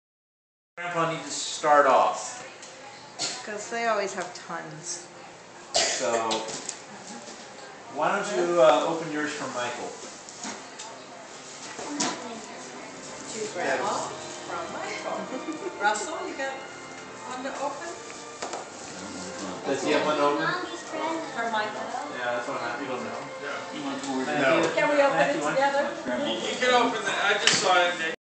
video-2010-12-28 Christmas with the Pimmel family
Opening presents with Grandma and Grandpa Pimmel, Tim, Dawn, kids and Joe.